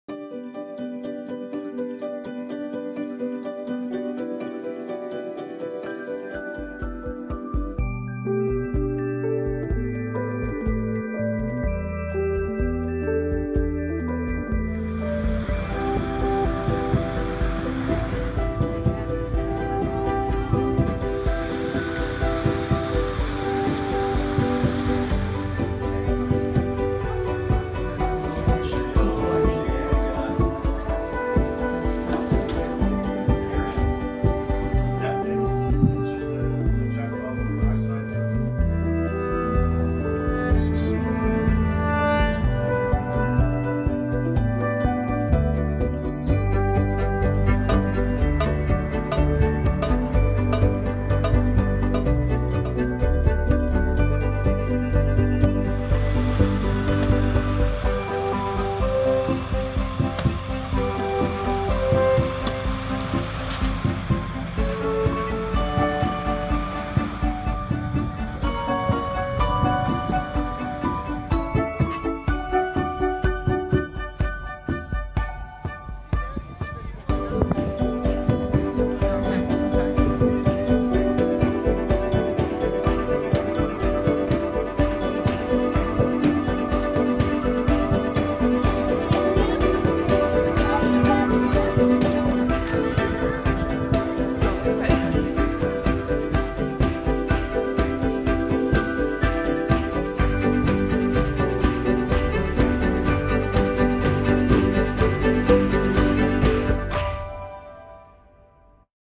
The Log Farm in Ottawa Ontario April 2 2017 Maple Sugar Bush Canada
Scenes from a visit to The Log Farm in Ottawa Ontario April 2, 2017.
The maple sap was running and maple taffy treats were being served.
Video Captured and edited on an iPhone 6 Plus.